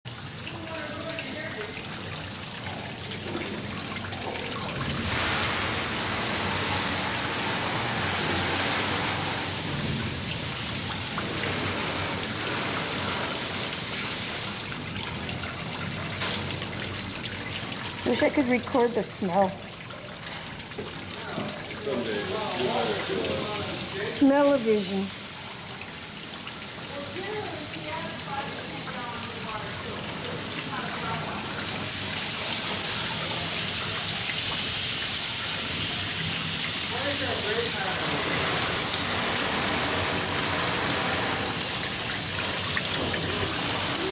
2008 Crush Press
Short clip of Zinfandel juice dripping from the press during Harvest 2008